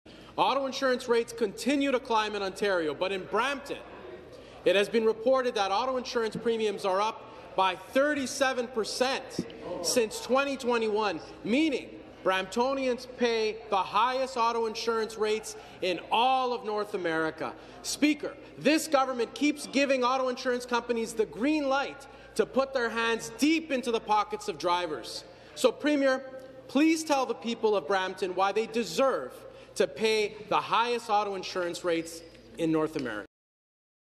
We deserve answers
Auto insurance rates continue to climb in Ontario,
but in Brampton, it has been reported that auto insurance premiums are up by 37%since 2021,meaning Bramptonians pay the highest auto insurance rates in all of North America.
Speaker, this government keeps giving auto insurance companies the green
light to put their hands deep into the pockets of drivers.
So premier, please tell the people of Brampton why they deserve to pay
the highest auto insurance rates in North America.